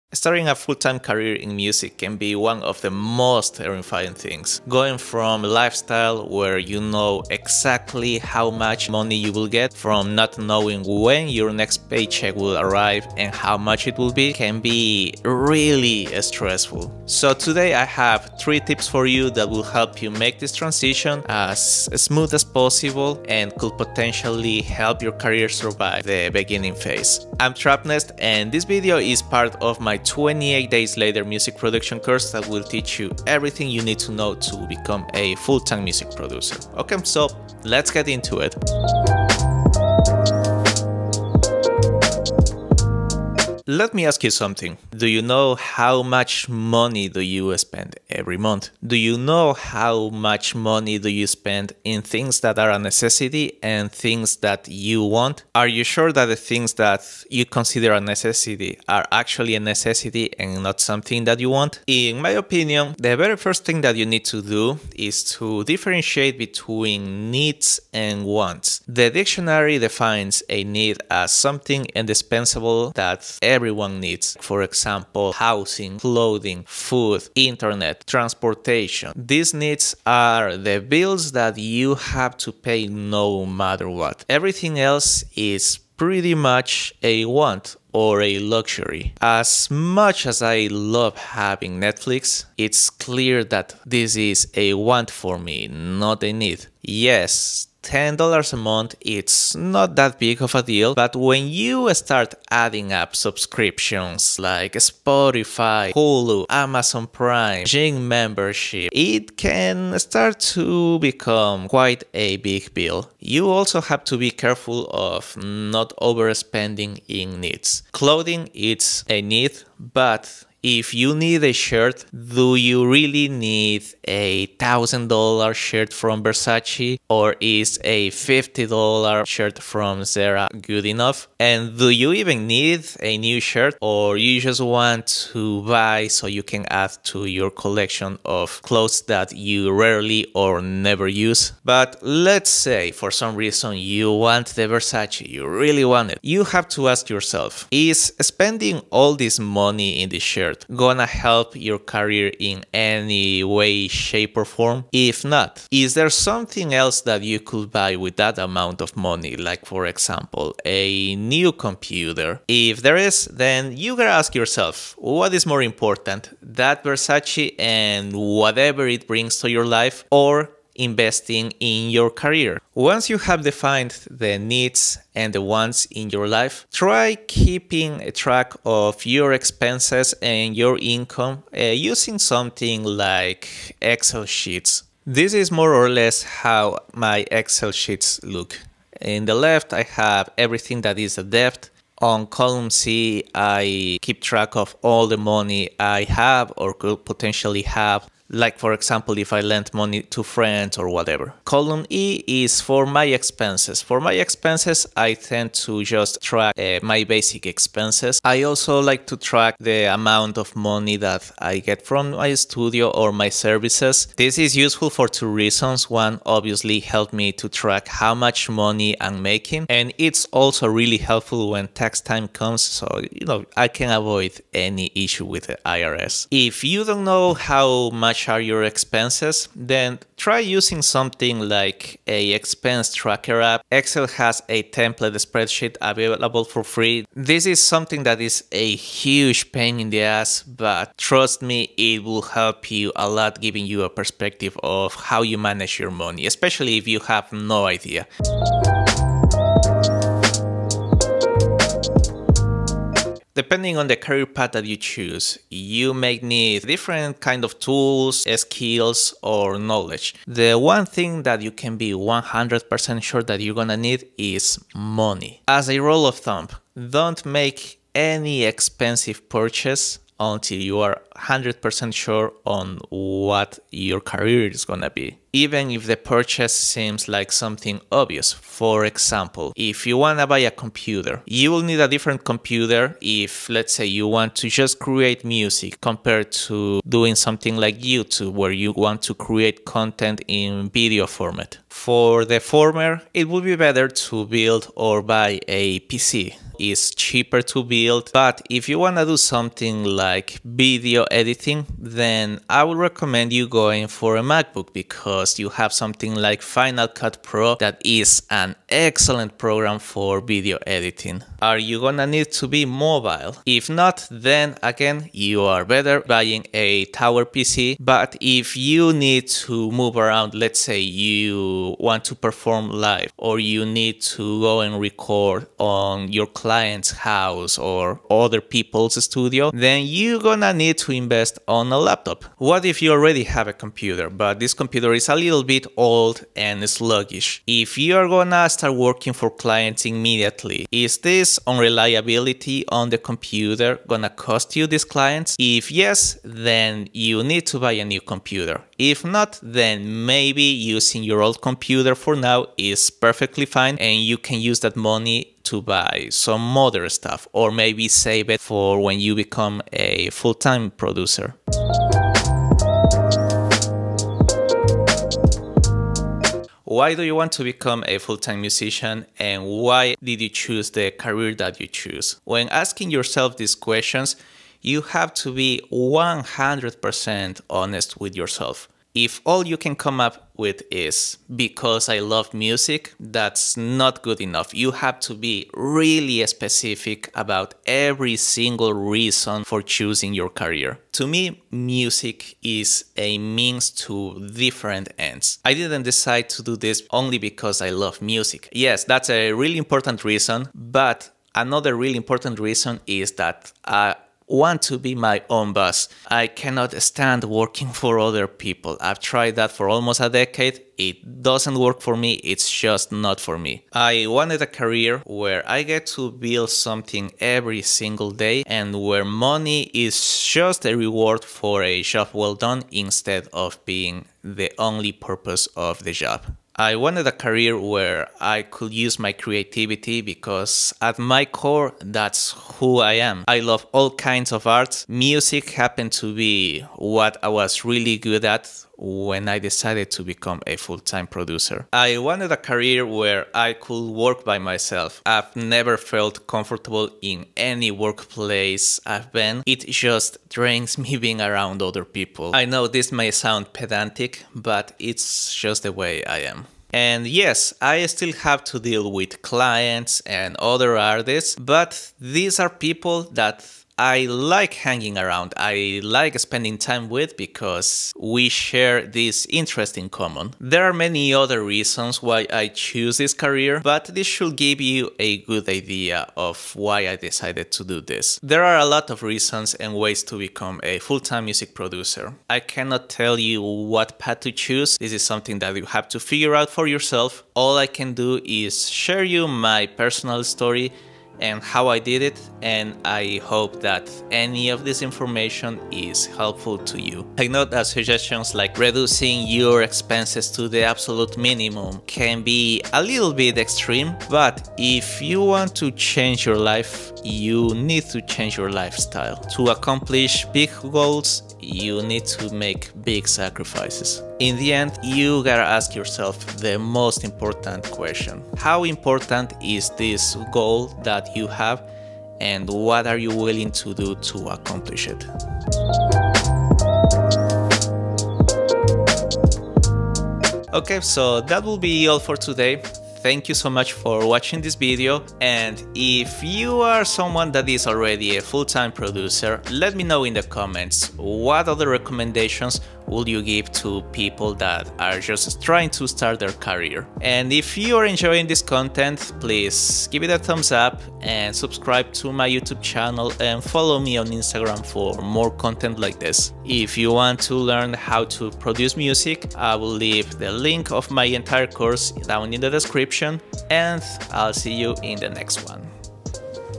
23-3 Things To Do Before Starting Your Music Business (FREE MUSIC PRODUCTION COURSE)
‼️ IMPORTANT INFO ‼️
This is the 23th of 28 videos on my "28 DAYS LATER" music production course that will teach you everything you need to know to become a FULL-TIME music producer.

In this video I give you what I consider are the 3 most important things any artist trying to become a full-time producer should know.

➤ SERVICES
1 on 1 coaching
Online Mixing
Online Music Production Lessons
Online DJ Lessons

- IGNORE THIS -